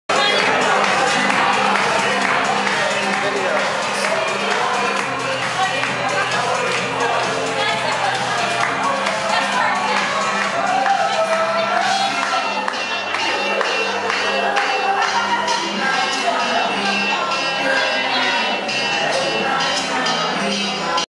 Singing Brittney
This is Brittney singing along in a karaoke bar at our hotel in the Dominican Republic. The people around here are some Canadians from Labrador that we met.